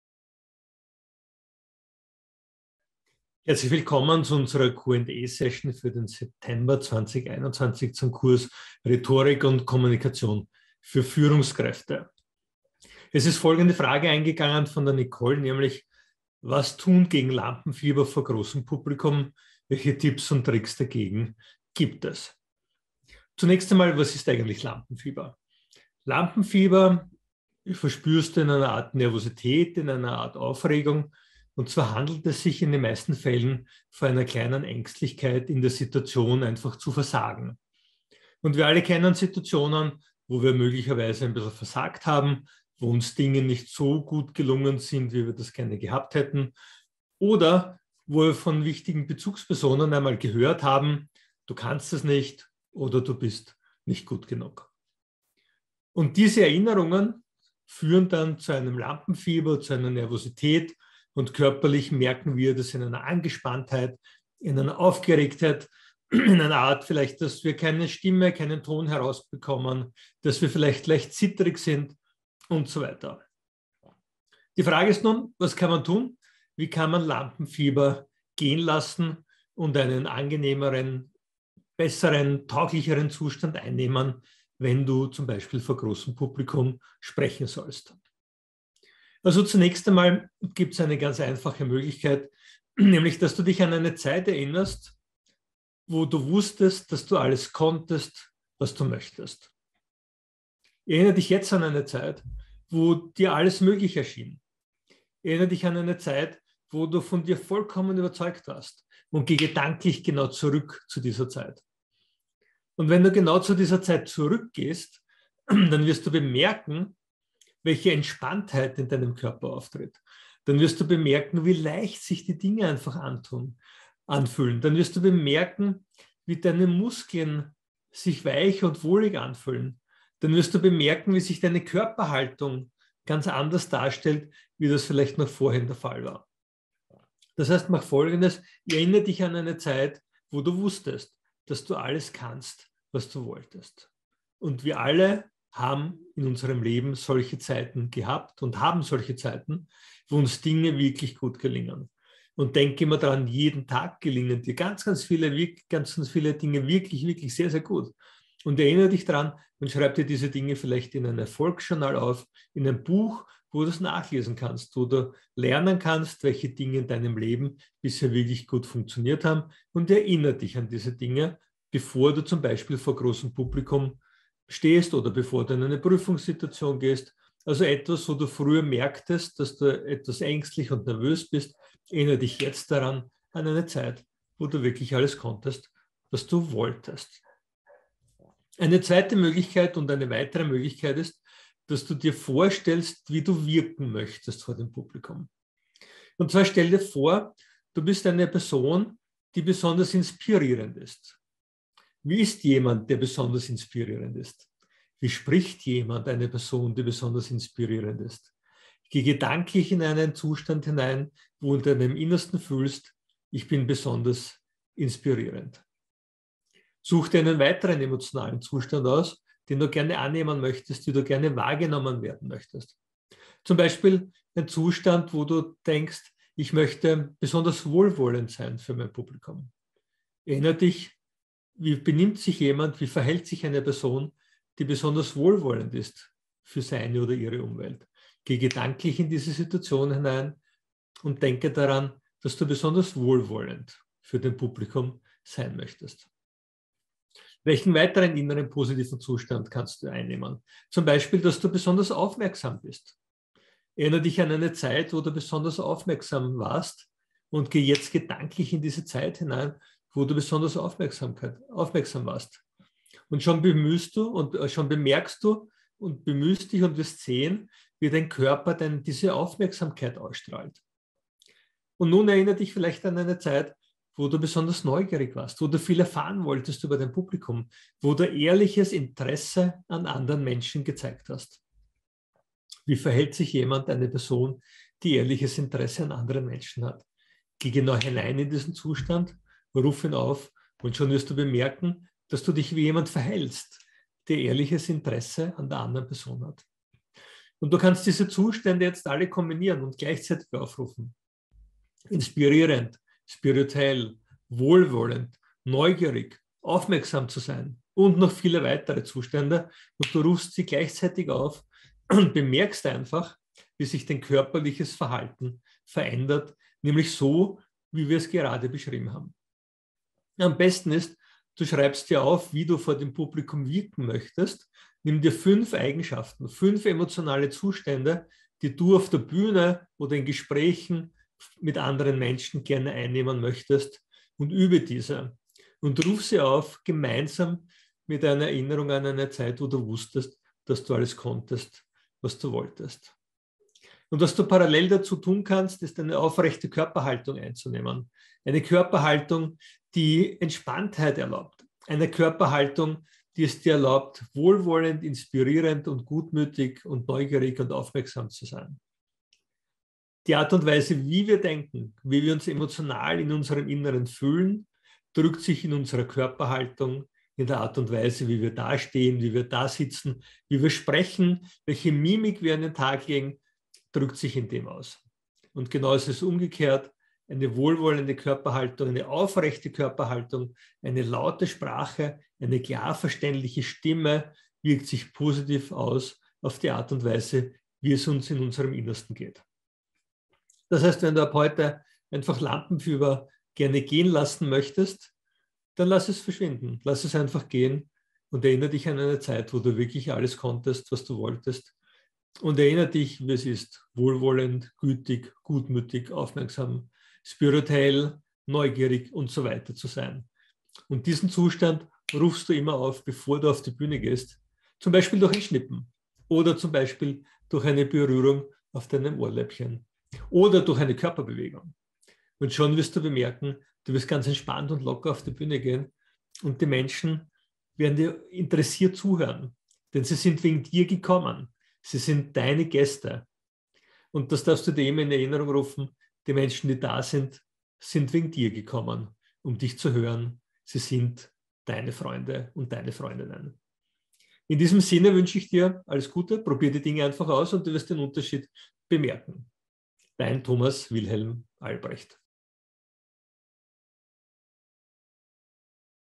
Rhetorik & Kommunikation für Führungskräfte | Wie kann man Lampenfieber gehen lassen?
*Thema:* Wie kann man Lampenfieber gehen lassen?
*Kurs:* Rhetorik & Kommunikation für Führungskräfte | Wissensvideo 7
*Dozent:* Thomas Albrecht

► 00:00 Intro
► 00:22 Was ist Lampenfieber?
► 01:10 Tipp 1: Erinnerung an eine  erfolgreiche Zeit
► 03:02 Tipp 2: Vorstellung, wie man in der Situation wirken möchte
► 05:24 5 emotionale Zustände üben
► 05:40 Körperhaltung
► 06:50 Wie kann man die Zustände aufrufen?

📖 *Deine Vorteile*
▸ Förderungen möglich